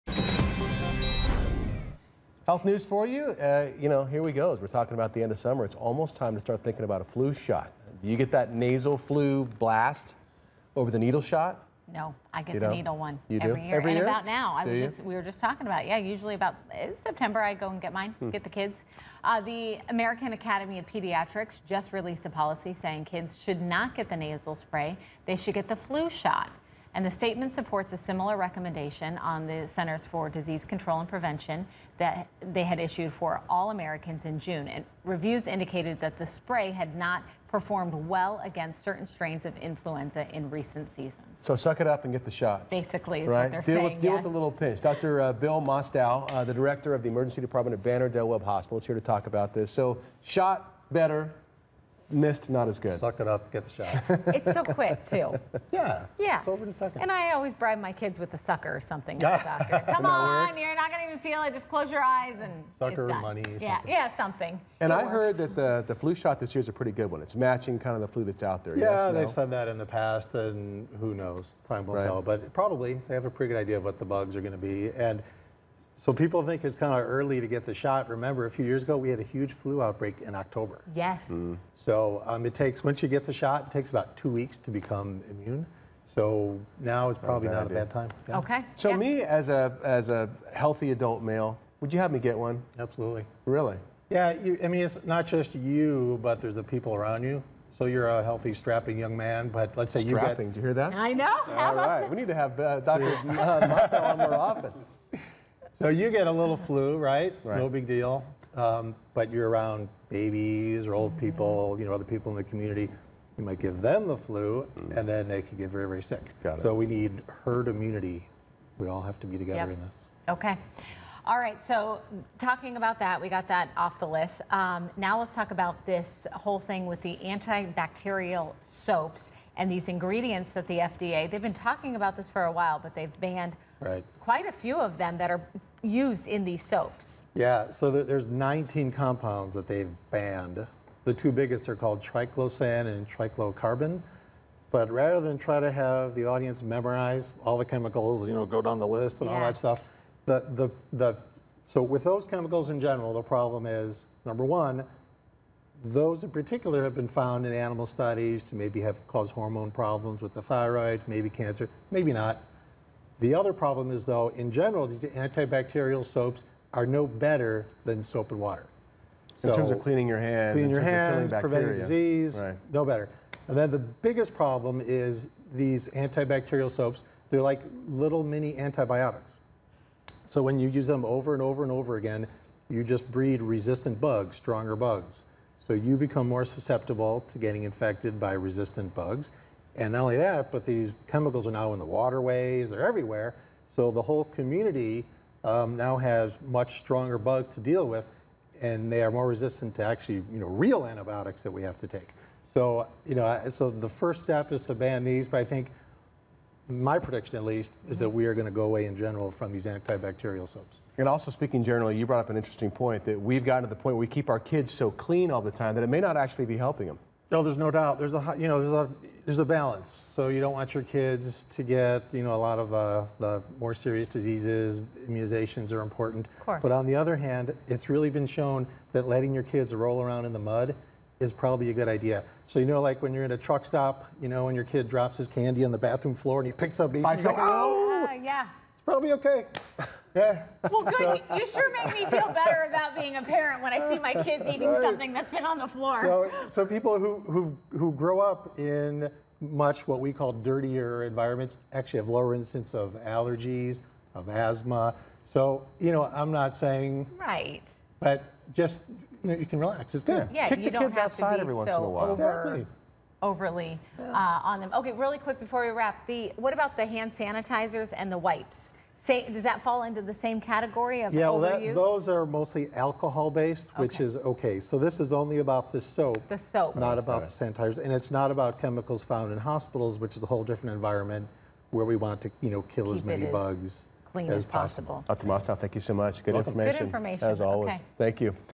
Flu shots and antibacterial soap
This year, the American Academy of Pediatrics is recommending all children get the needle flu shot, not the nasal spray. Doctors say the spray does not perform well against certain strains of influenza. Dr. Bill Mostow, the Emergency Room Director at Banner Del Webb Hospital talks about the best time to get the flu shot and about the FDA’s recent ban on 19 ingredients commonly found in anti-bacterial soaps.